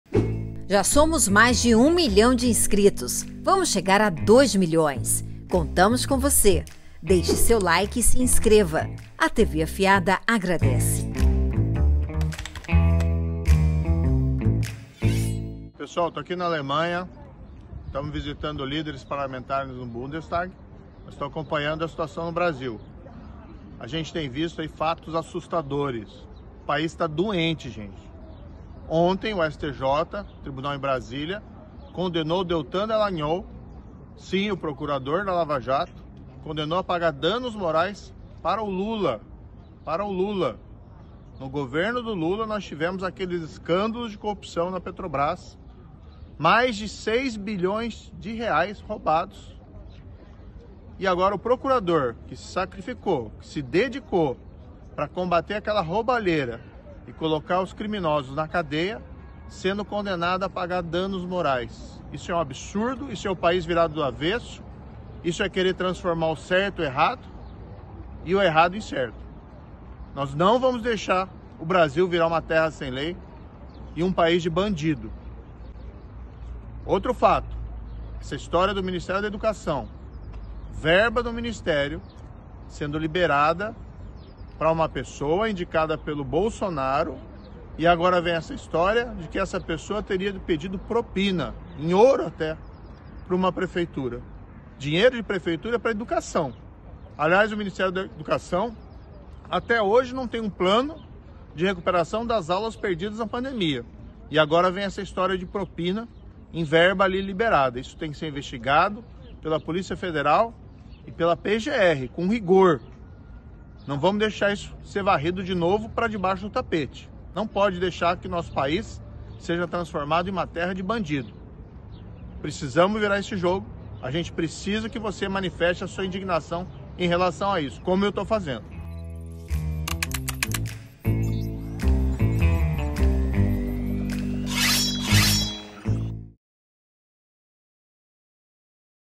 MORO DEFENDE DALLAGNOL, ATACA LULA, STJ E COMENTA SOBRE BOLSOLÃO DO MEC | 1KG DE OURO | BOLSONARO
O ex-juiz e ex-ministro da Justiça Sergio Moro divulgou nesta quarta-feira (23) um vídeo em que ataca o STJ por condenar o ex-procurador Deltan Dallagnol a indenizar Lula por danos morais no caso do PowerPoint. Moro também cobrou uma investigação sobre um suposto esquema de favorecimento no MEC, que teve o nome do presidente Jair Bolsonaro citado.